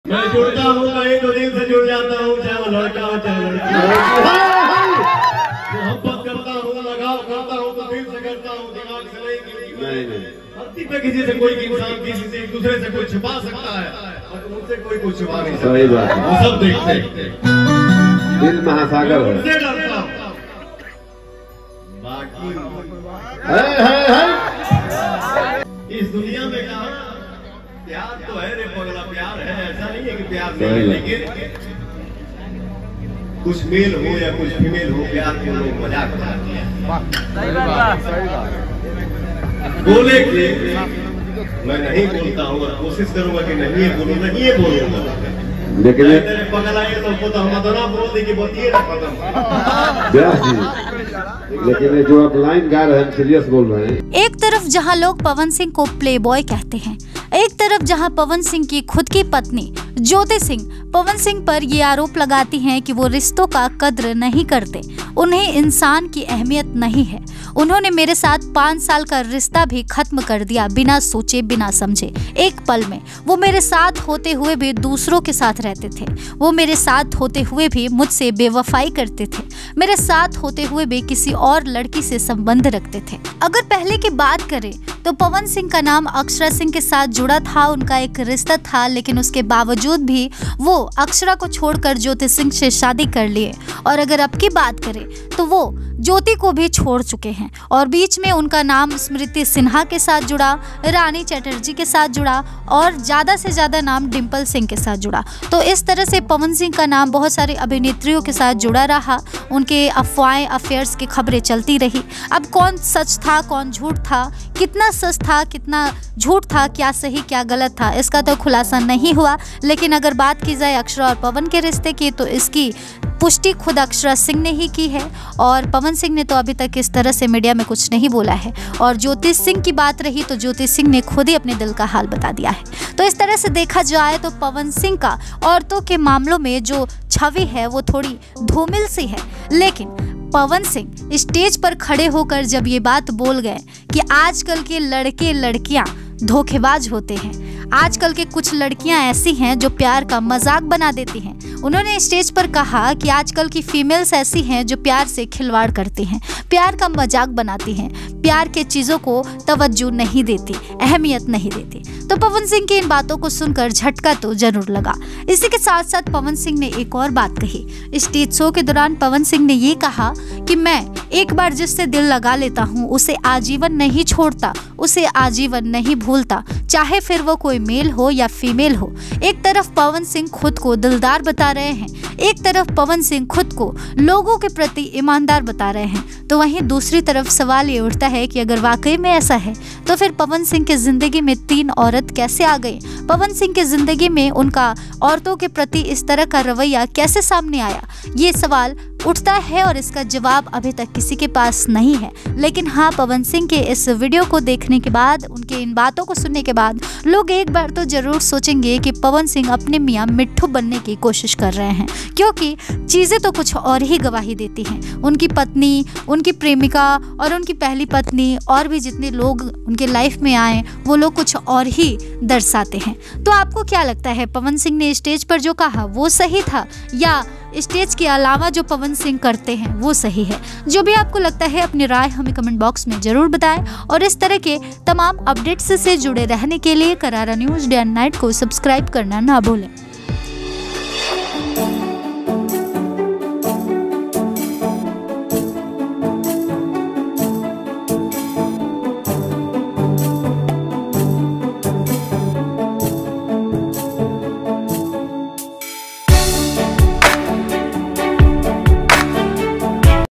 Pawan Singh Stage Show : पवन सिंह को किसने दिया धोखा ? जानिए क्यों स्टेज पर दिल का दर्द किए बयां ?
Channel - Karara News Day & Night
Contents - News & Informational
Language - Hindi
Location - Uttar Pradesh
News Anchor - Virat Bhardwaj & Shweta

दोस्तो _

Channel Motive -

VIDEO TITLE :-Pawan Singh Stage Show : पवन सिंह को किसने दिया धोखा ? जानिए क्यों स्टेज पर दिल का दर्द किए बयां ?

VIDEO DETAIL :- Pawan Singh Latest News